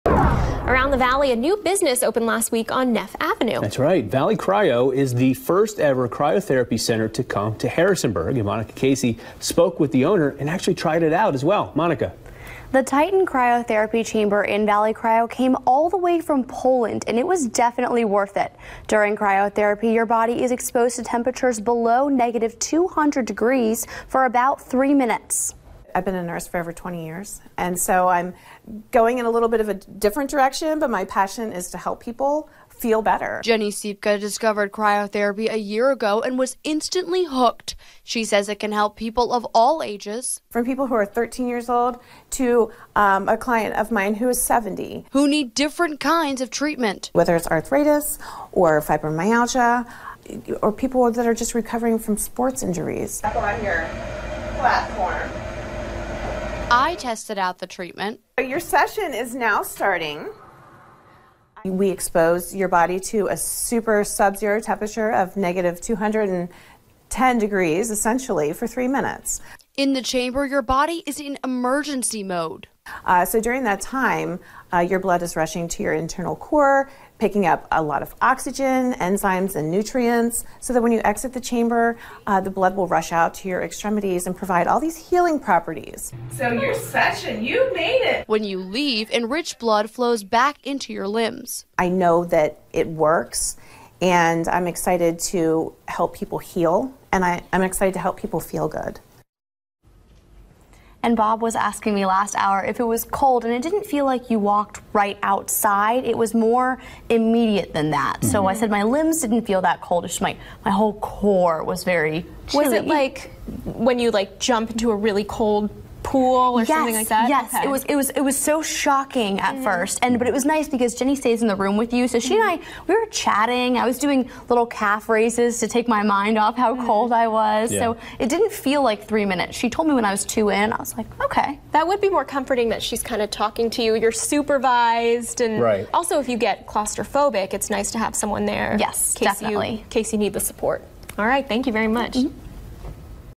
Cryotherapy center opens in Harrisonburg
The first-ever cryotherapy treatment center opened in Harrisonburg, and I tried it out!